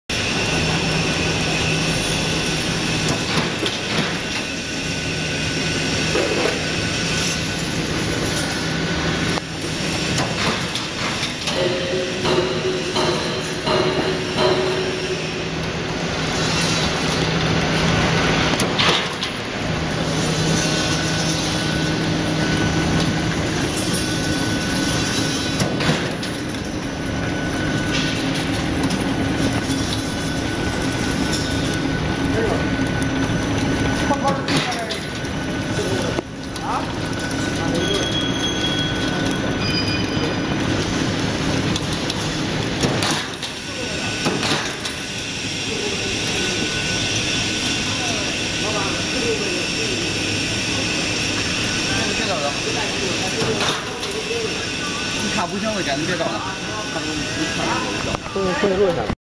One Strips Razor Barbed Wire Machine straitening and welding machine
Razor barbed wire machine one strips,can straitening and cutting ,then welding razor wire mesh.